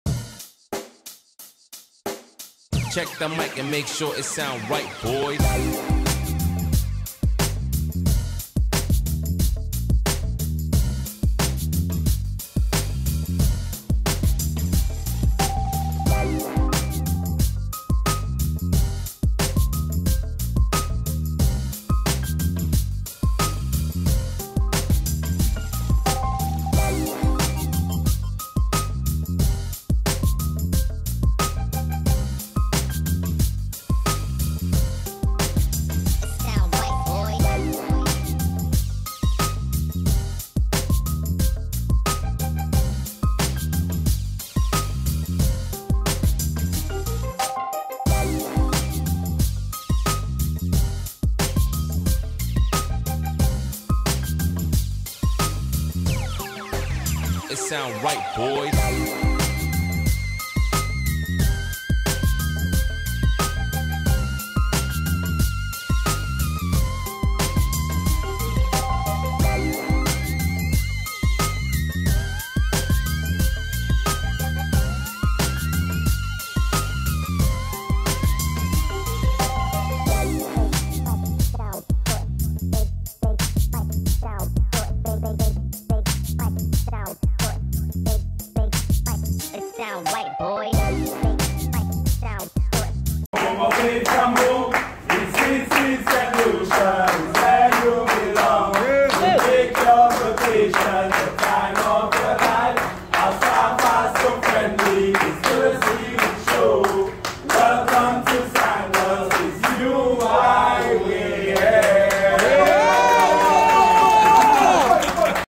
SANDALS AIRPORT ARRIVAL TO SANDALS REGENCY LA LOC RESORT PT 2
This video shows the area of the airport that Sandals has for anyone that going to there resorts. It also shows clips of our transport to the @SandalsResorts